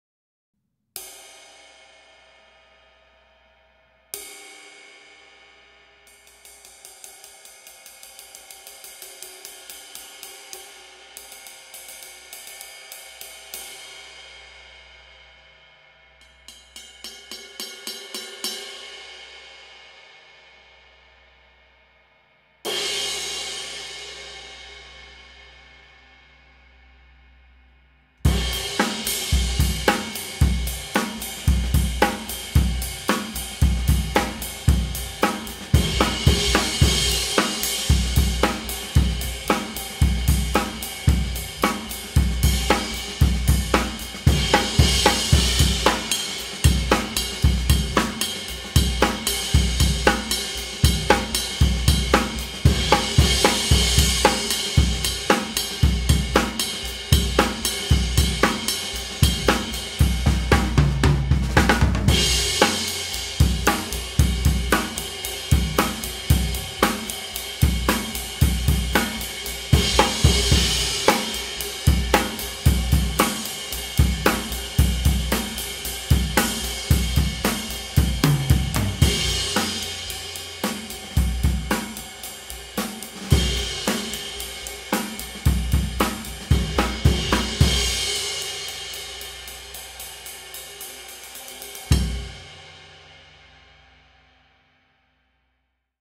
Used Istanbul Agop 21" Traditional Original Ride Cymbal - 2603g (UORR21-1072222M)
SOLD - Used Istanbul Agop 21" Traditional Original Ride Cymbal

Other gear used in this video:
• Cymbals (From drummer's L to R): Used Istanbul Agop 14" Traditional Medium Hi-Hats, Used Istanbul Agop 20" Agop Signature Ride Cymbal